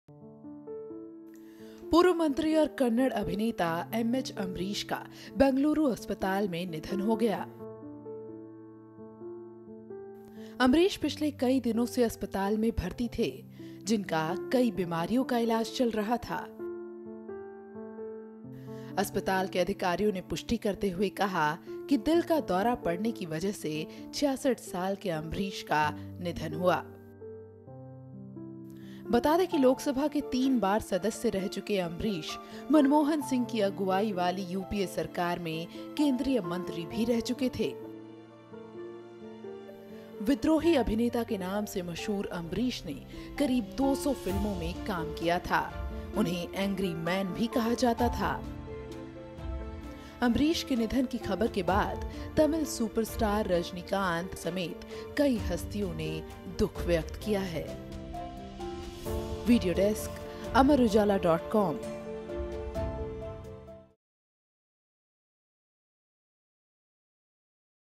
Karnataka kannada actor congress leader ambareesh passes away due toheart attack
अभिनेता से नेता बने पूर्व केंद्रीय मंत्री अंबरीश का दिल का दौरा पड़ने से निधन हो गया। 66 साल के अंबरीश पिछले कई दिनों से अस्पताल में भर्ती थे। उनका कई बीमारियों का इलाज का चल रहा था।